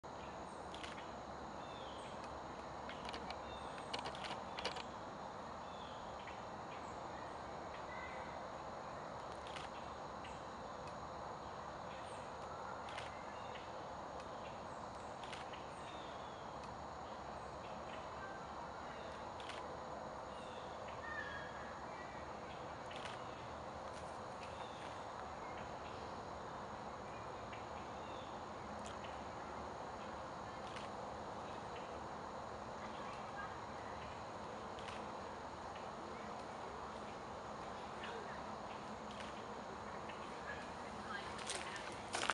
Cooper's Hawk with prey
Blacklick Woods (near Columbus, Ohio)
09/27/11
No tripod and shot through a 300mm lens.  It is a bit unsteady and at times out of focus.  However,  it is rare for me to see this.